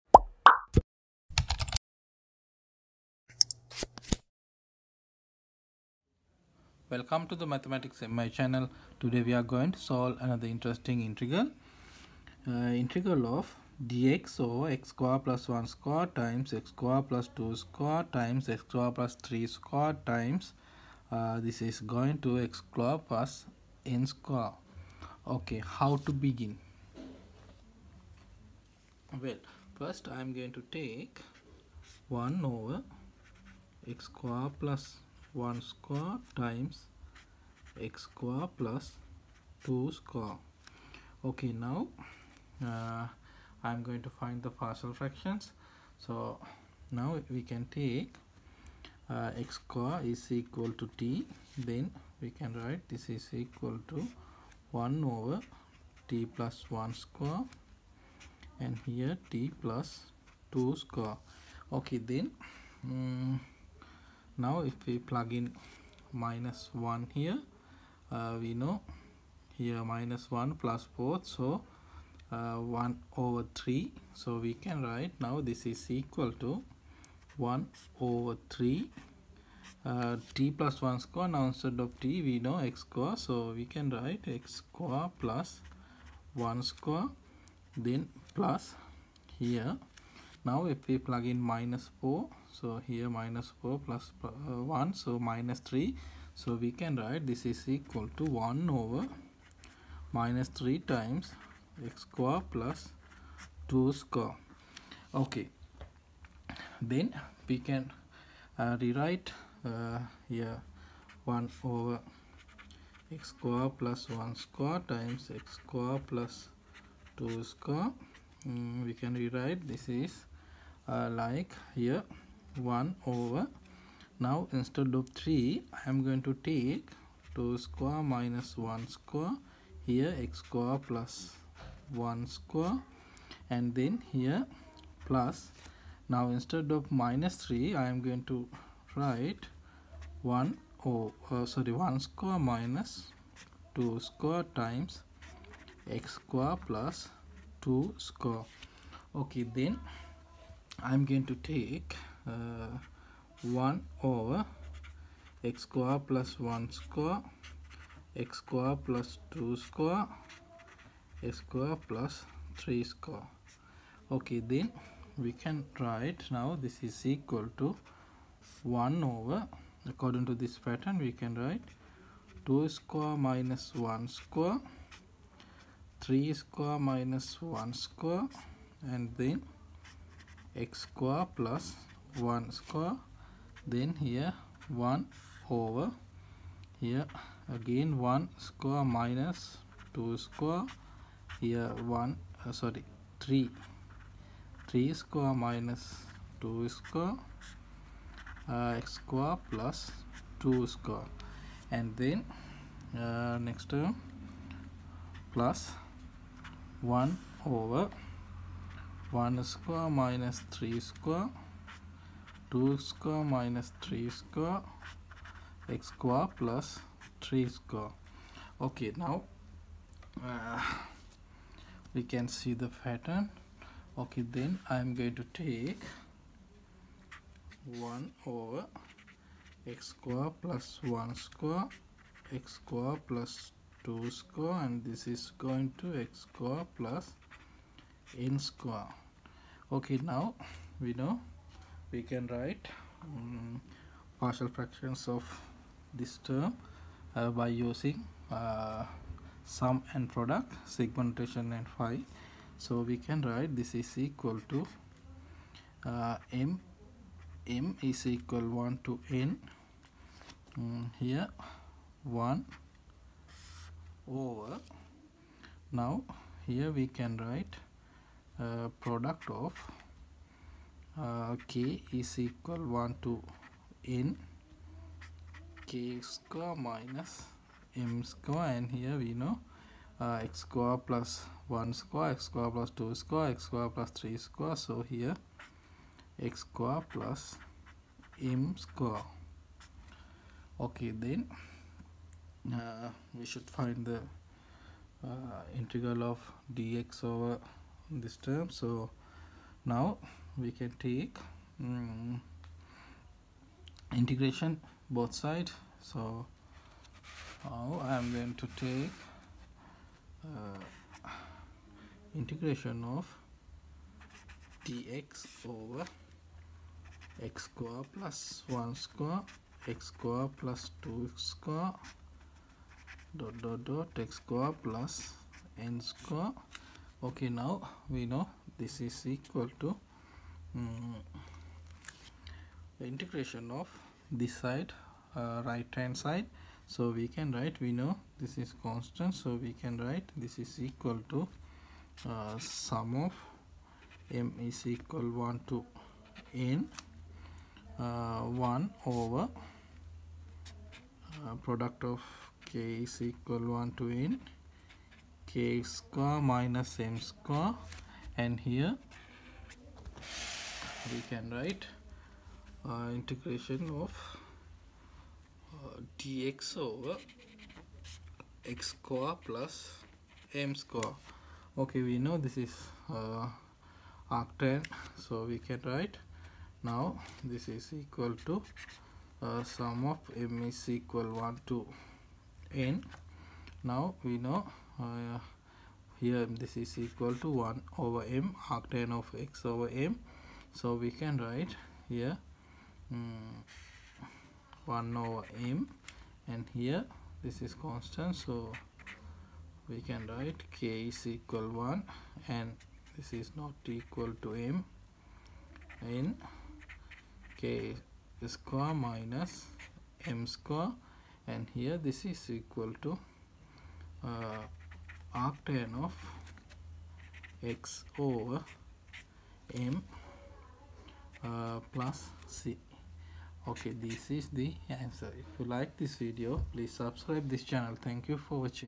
Interesting Indefinite Integral
This video explains how to solve this interesting indefinite integral.
Indefinite Integration
Indefinite integrals
integration
Hard integral
Hard integral problems
Integral calculus